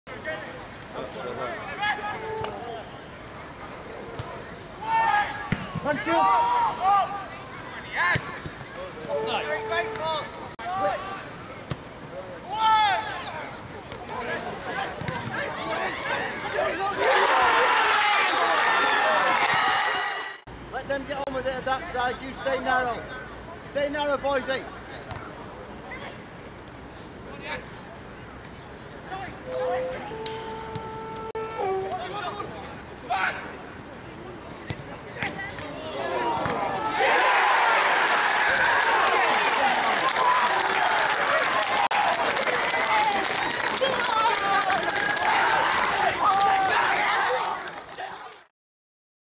Under-18s | Sheffield Wednesday 1-1 Sheffield United | 2018/19
Liam Shaw’s stoppage time equaliser earned the Owls Under-18s a priceless point in the Sheffield derby on Saturday morning.

In a dramatic and entertaining U18 Professional Development League contest, Sheffield United took the lead 15 minutes from time through Jean Belehouan.

But as time ticked down on the derby, the Owls rallied, and their efforts were rewarded in stoppage time when Shaw bundled home the ball from close range